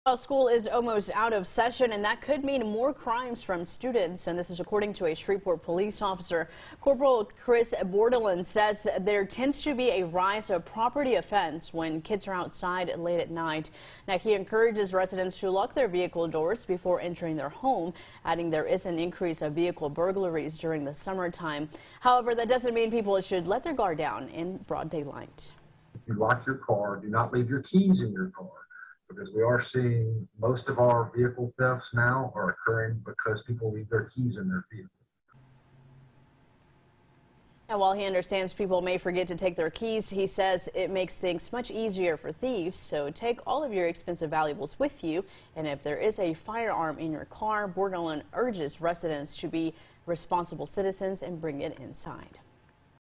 SPD warns residents to avoid property crime
Summer is approaching which means longer days, warmer temperatures, and the potential for an increase in juvenile crime according to the Shreveport Police Department.